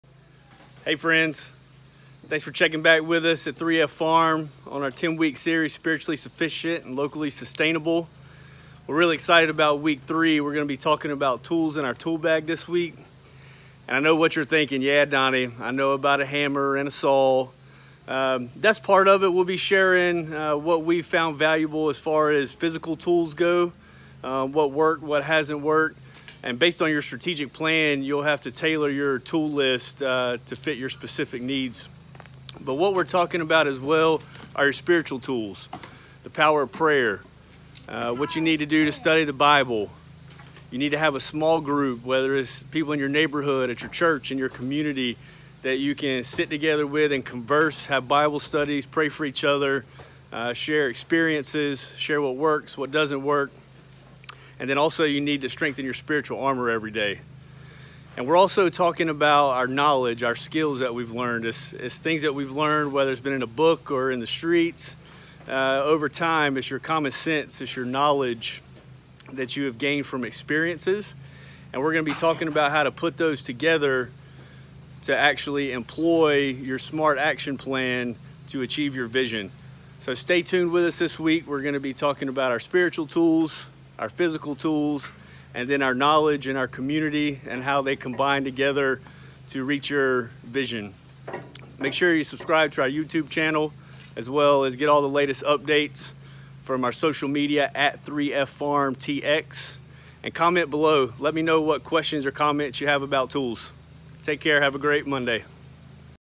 WEEK 3- Tools in the Toolbag
We all need specialized tools to achieve our unique sustainable strategies, and this week we will discuss how to stock your Toolbag to successfully tackle the SMART action plan we developed last week.